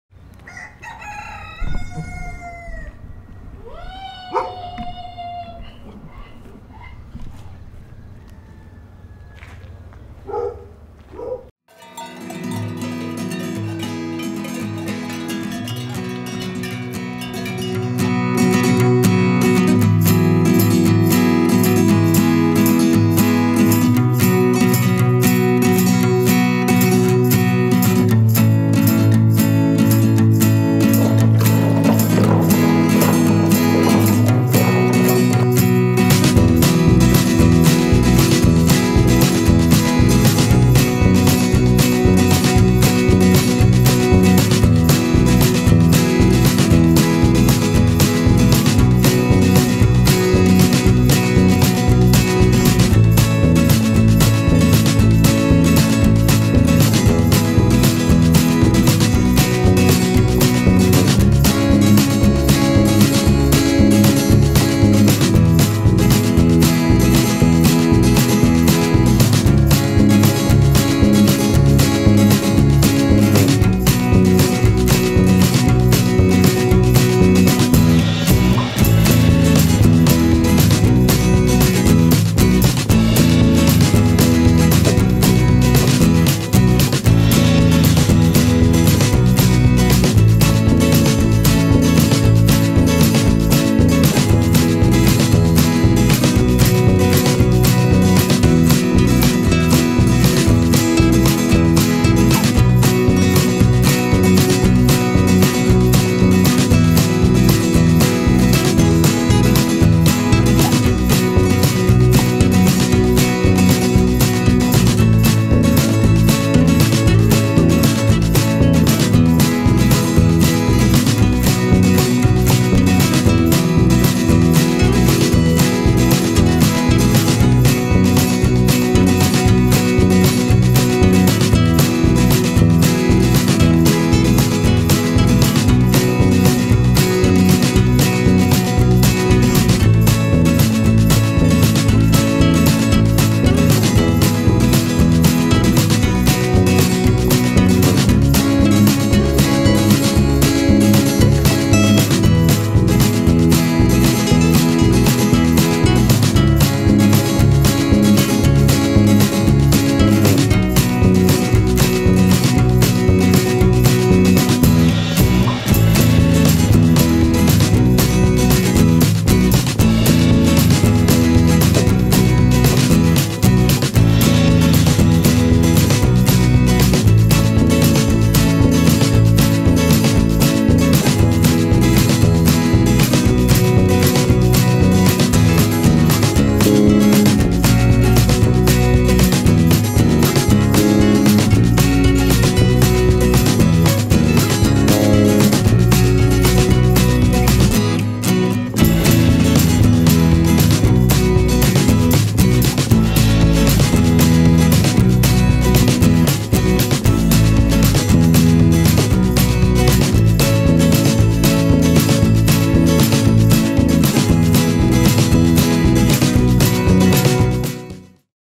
Il Caciocavallo di Godrano
Siamo in Sicilia, nella provincia di Palermo, in un paesino di nome Godrano, dove si produce un eccellente caciocavallo.